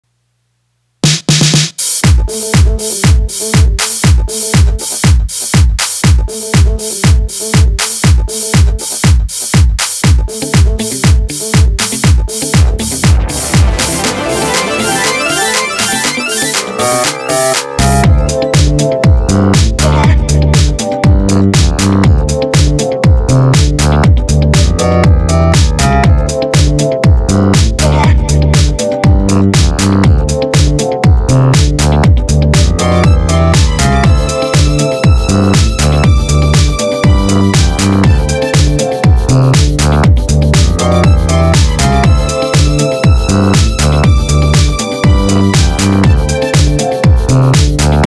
Immigration problems in the past.
Illegal immigration is not a new problem. See how it played out many many rears ago.